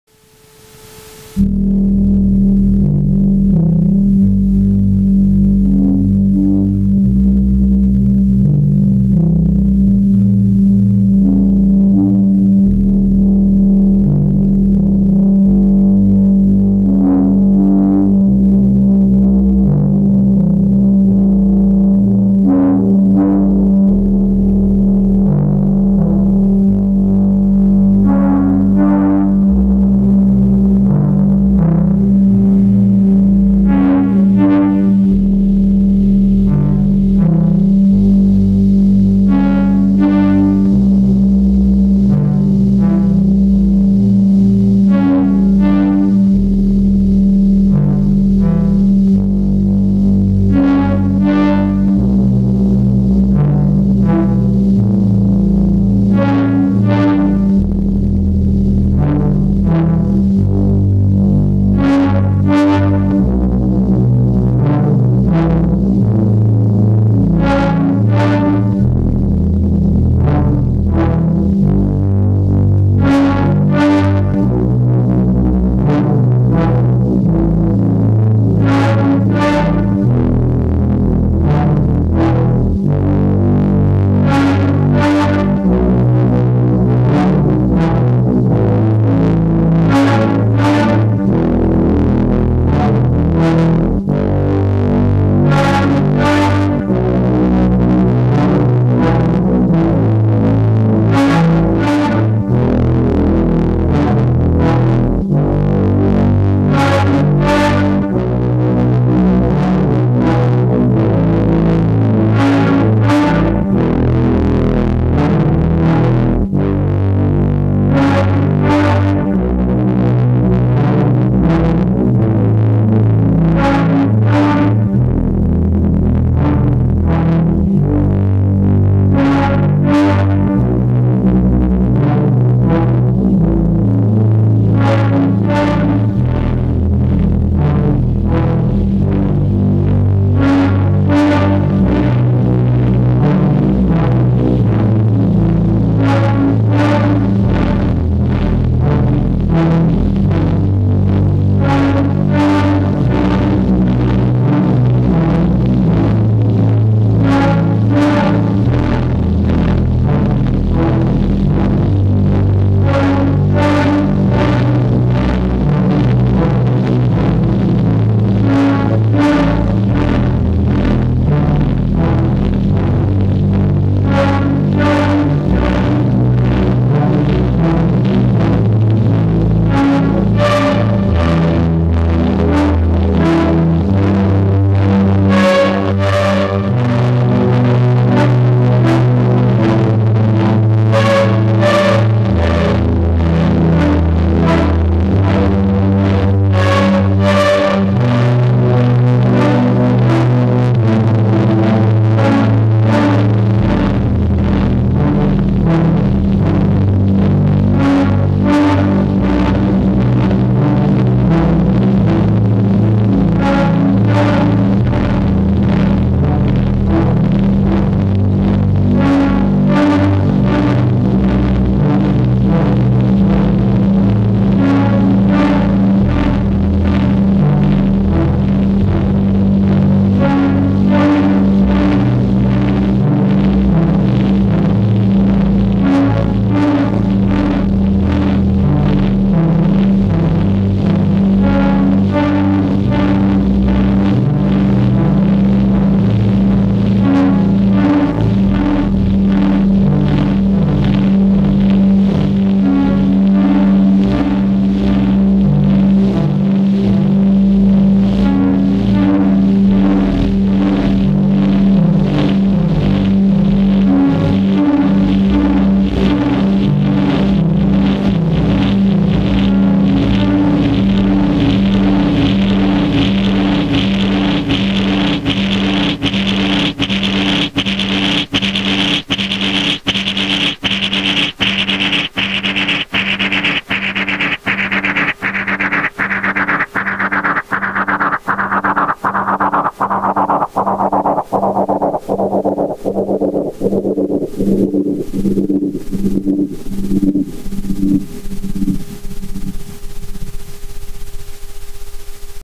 Just noodling, in bed, with the 3 voice polyphony of the volcaFM, & ran it thru a MonotronDelay
Damn, but I fall mor in love with these little volcas, all the time!! Limited...definitely. ”toys”. no way!! You rule Korg!